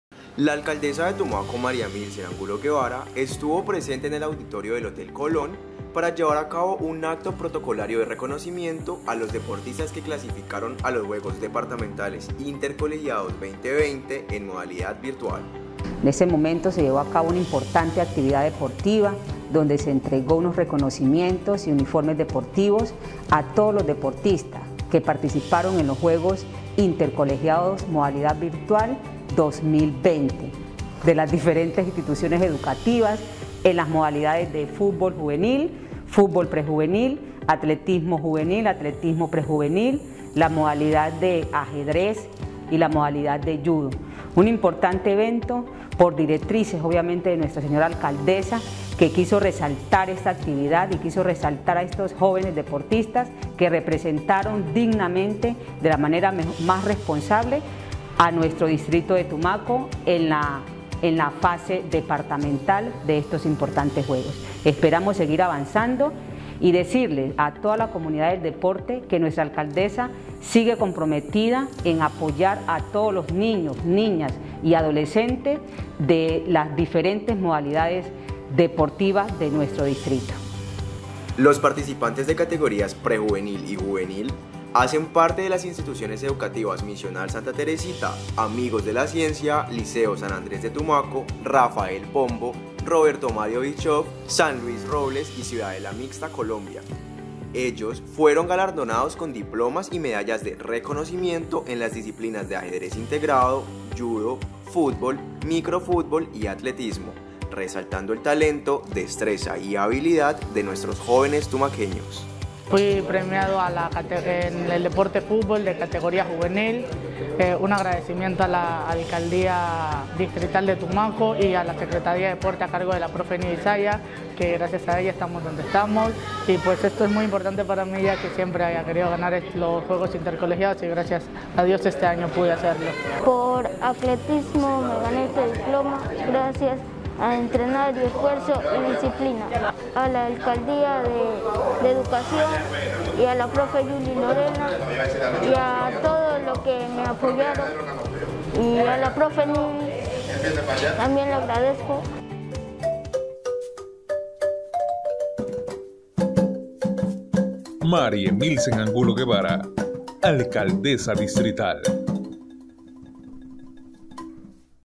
ALCALDESA GALARDONÓ A DEPORTISTAS QUE CLASIFICARON A LOS JUEGOS DPTAL. INTERCOLEGIADOS 2021
La Alcaldesa de Tumaco María Emilsen Angulo Guevara a través de medallas y diplomas de reconocimiento, exaltó el esfuerzo y talento de los deportistas clasificados a los Juegos Departamentales Intercolegiados 2021 en modalidad virtual.